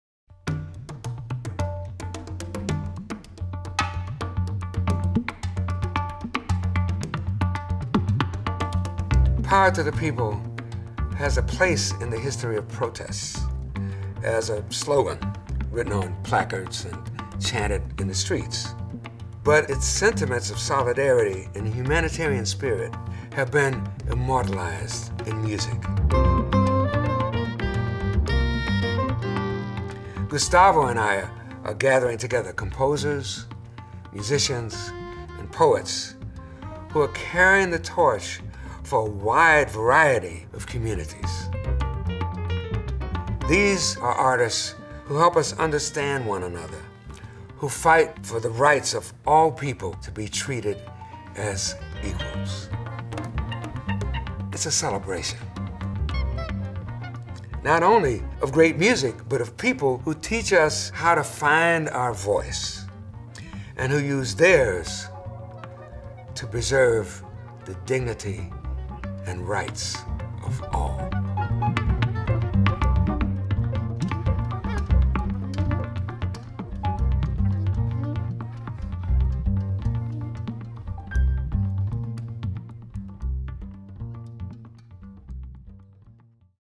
Herbie Hancock Introduces the Power to the People! Festival
Herbie Hancock, LA Phil Creative Chair for Jazz, speaks about the Power to the People! Festival, a program of concerts, discussions, and events celebrating the role artists have played in advancing social change, civil rights, and humanitarian causes.

Power to the People! Festival
MAR 5 - APR 11